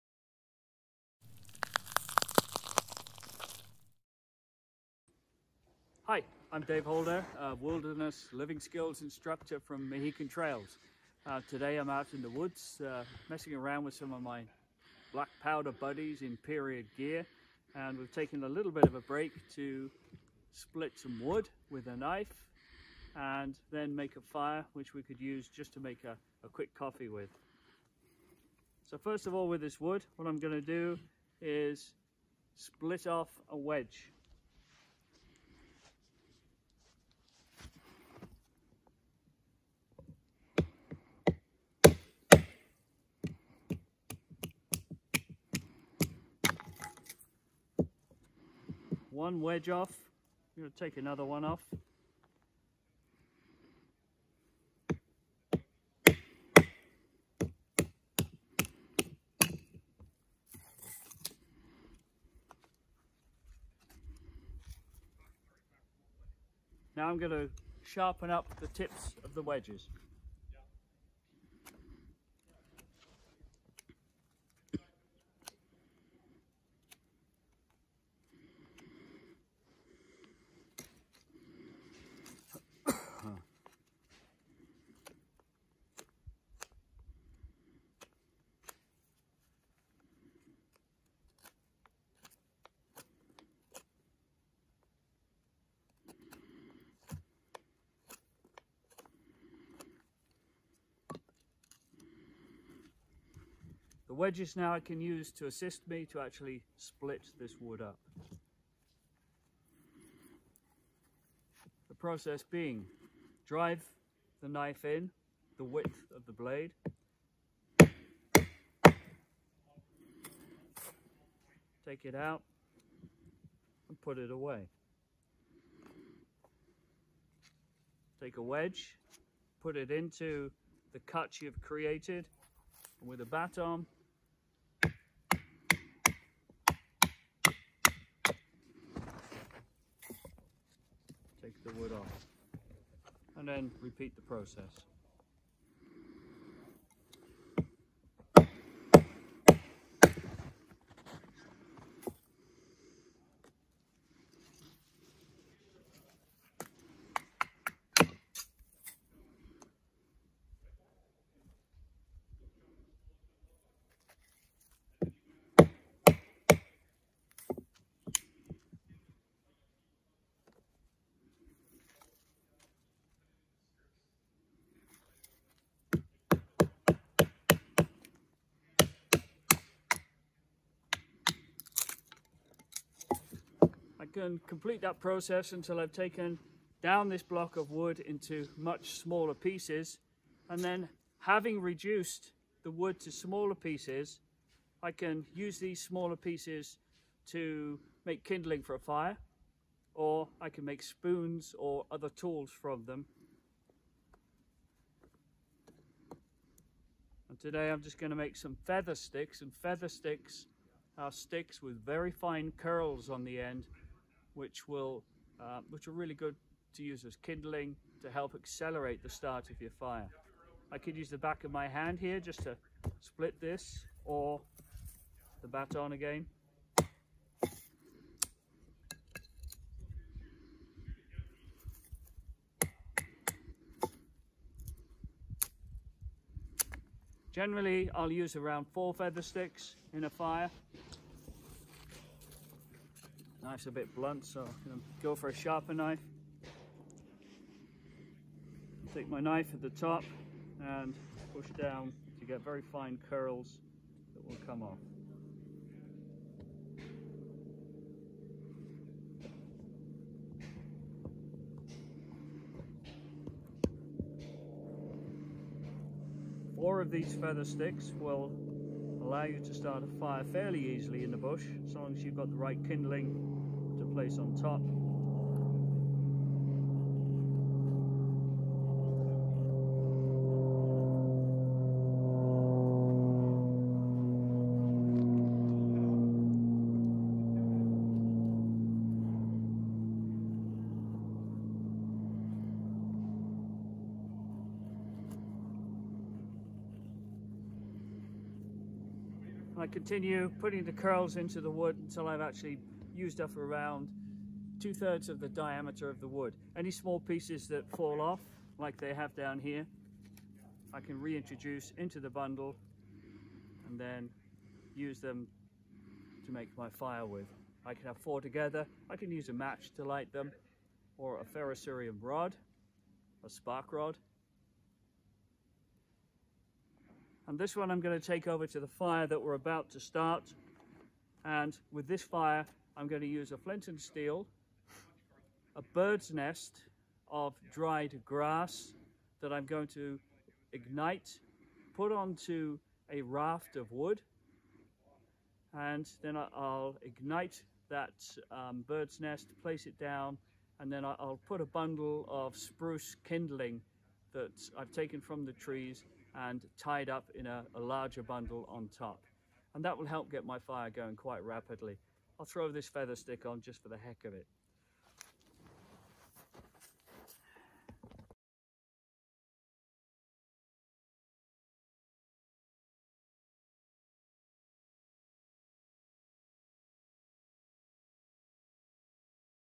Survival Video_How to build a fire
Sundre Museum's - Survival Video Series.

In this video you will meet Dave Holder. He taught his first official survival lesson while serving in the British Army back in1984. Initially trained as a Combat Engineer he gained extensive training as aMountain Leader where he led groups of soldiers in numerous countries from Africa, across Europe, Central America and the USA. Dave currently works with his wife’s company ‘Mahikan Trails’ based near Sundre, Alberta, teaching outdoor skills, plant medicine, first aid, and survival bushcraft skills. For more information you can find him